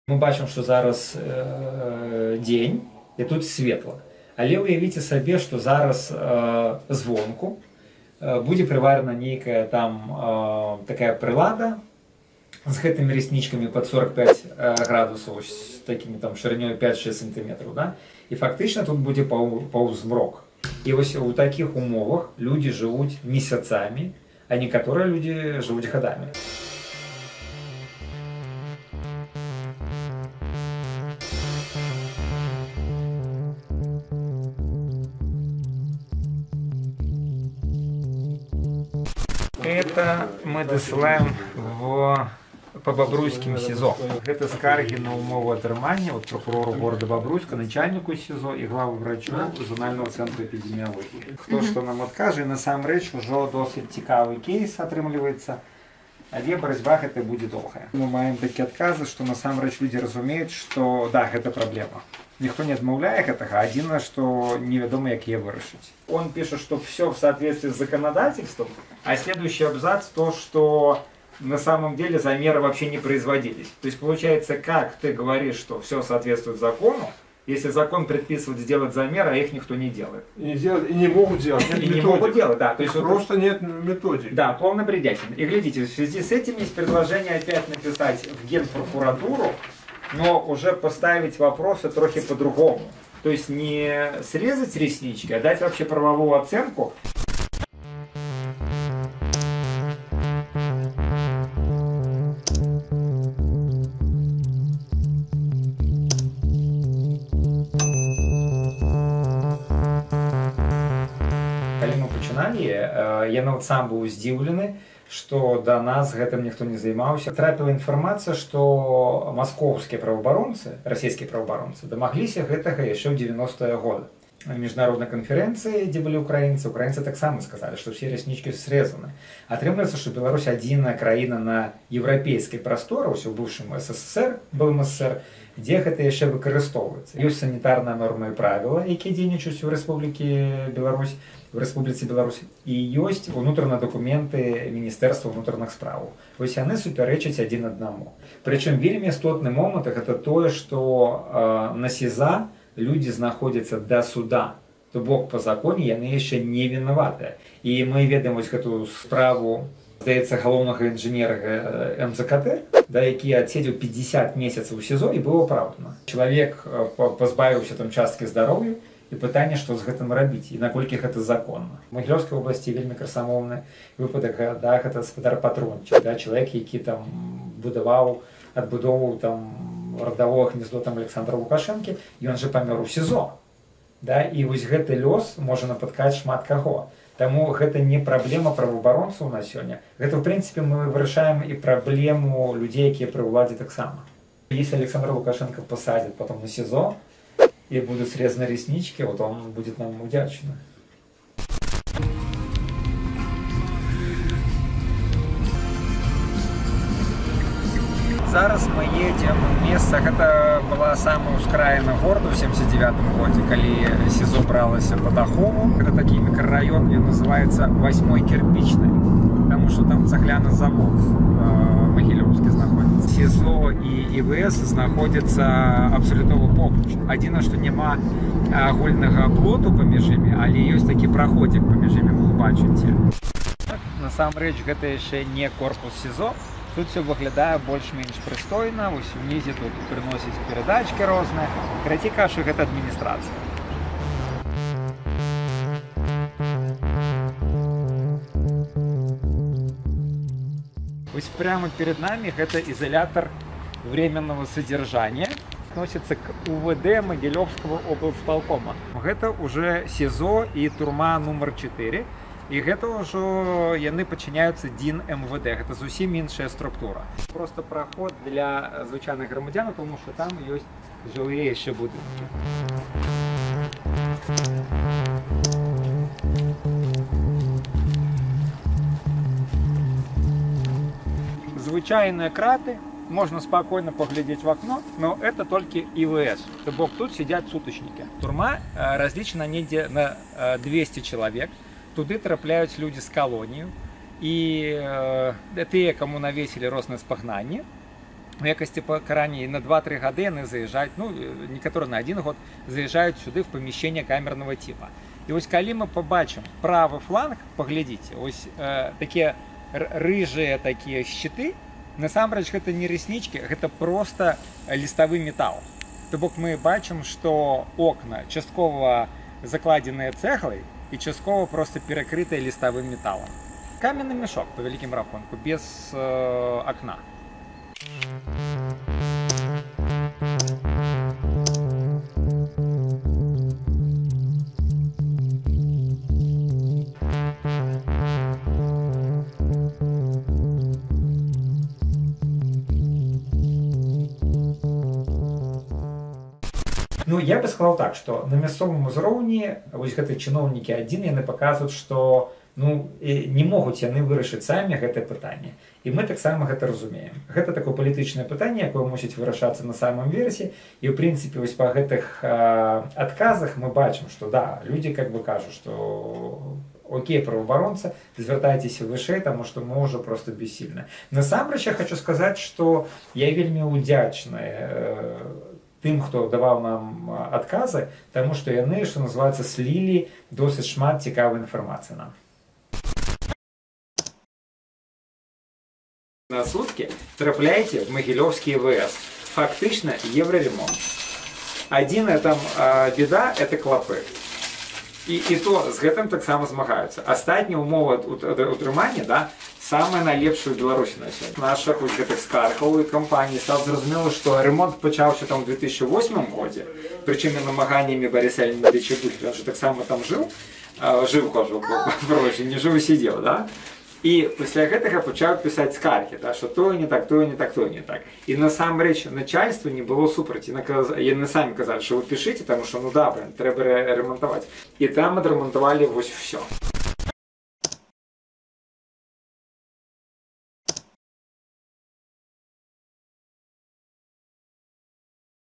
Раснічкі. Чаму ў турме мала святла / Реснички. Почему в тюрьме мало света
Чаму ў турмах паўзмрок і амаль няма натуральнага святла?
Магілёўскія праваабаронцы "Вясны" распачалі кампанію супраць “раснічак” на турэмных вокнах. Менавіта наяўнасць гэтых "раснічак" перашкаджае святлу трапляць у турэмную камеру. У турмах Беларусі да цяперашняга часу выкарыстоўваюць "раснічкі"